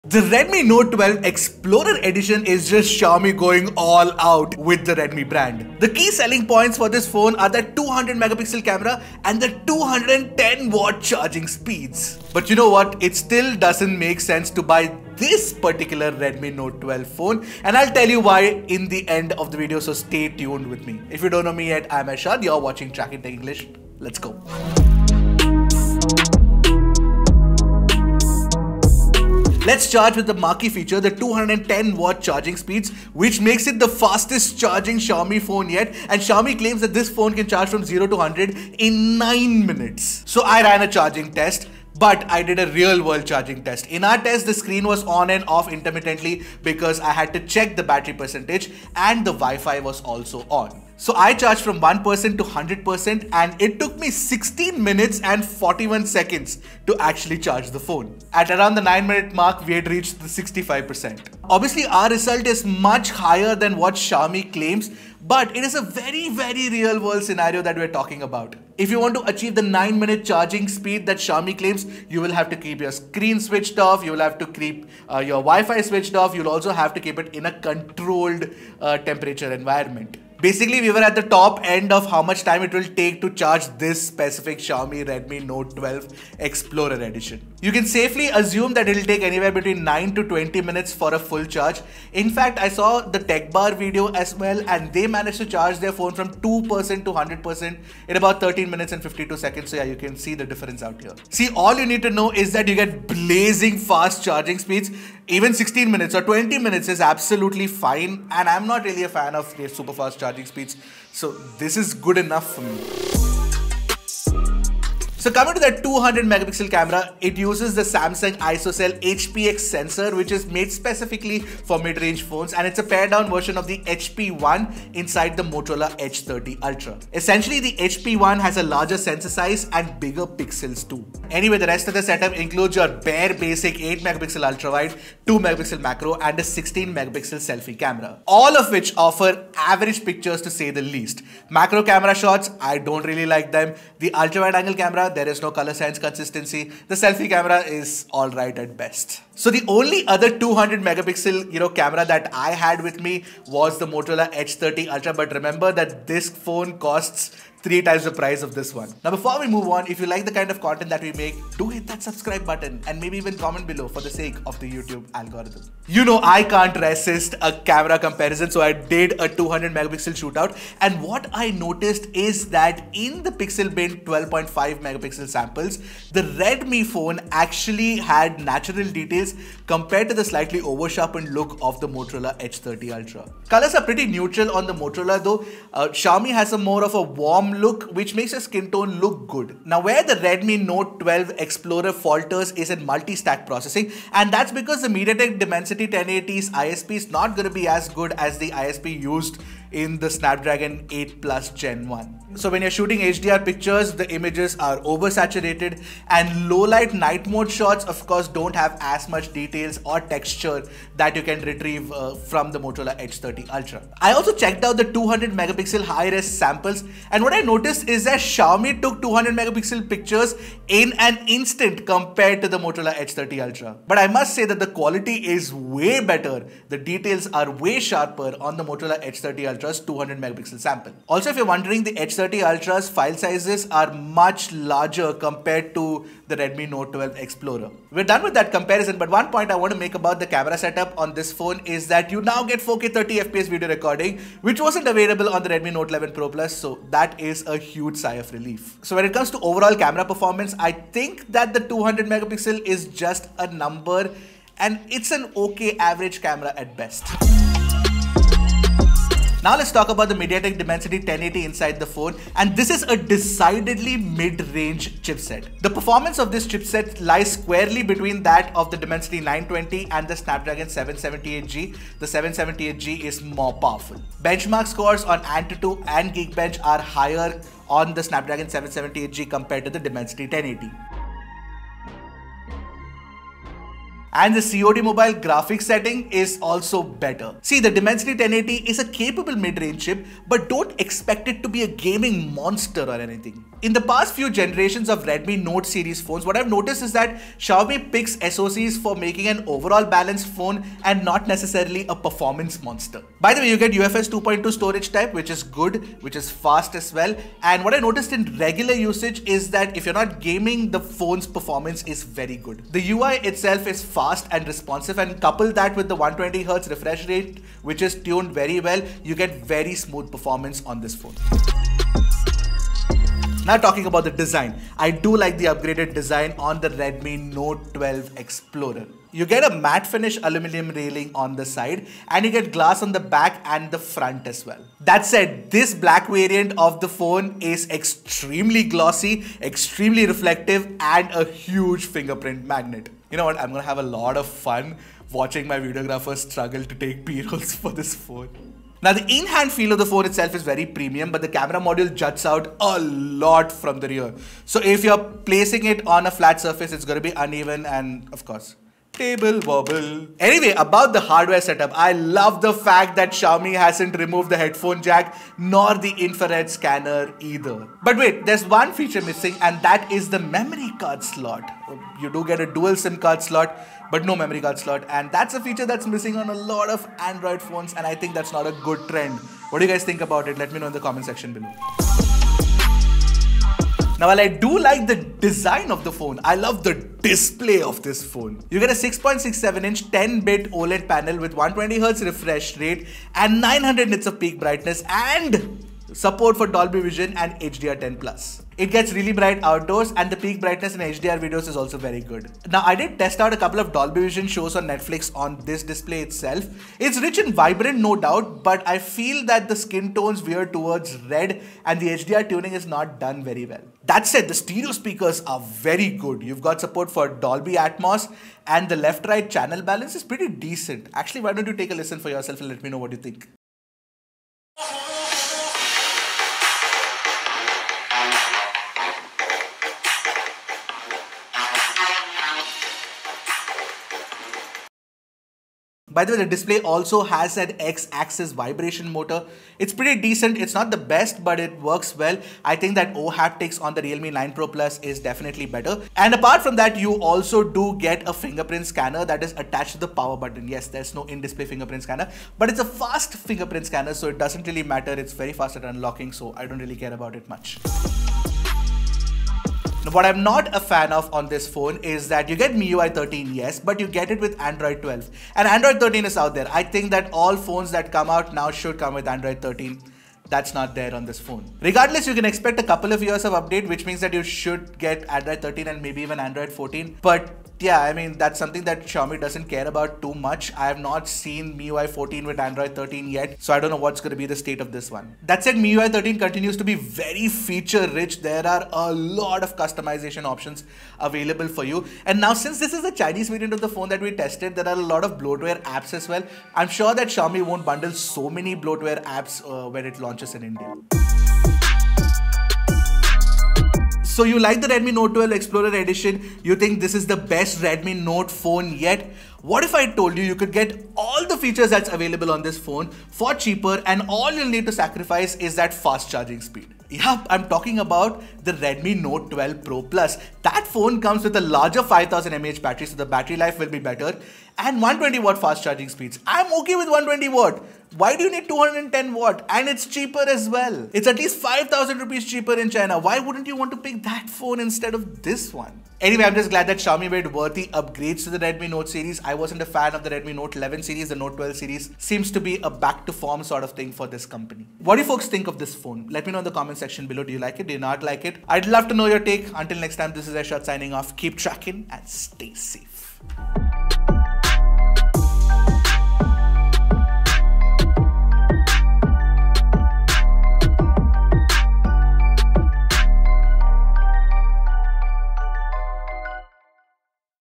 Redmi Note 12 Explorer - Best, Most Expensive Redmi Note But There’s a Hiccup!
We imported the latest Redmi Note 12 phone and the most expensive one at that: the Redmi Note 12 Explorer Edition. Xiaomi has made the right kind of changes to the phone compared to the previous Redmi Note 11 series but, the Explorer Edition has one problem...

Video Highlights
00:00 Intro
00:27 210W Charging
01:52 Camera
04:17 Processor
05:21 Design
06:21 Display
07:37 Software
08:20 Conclusion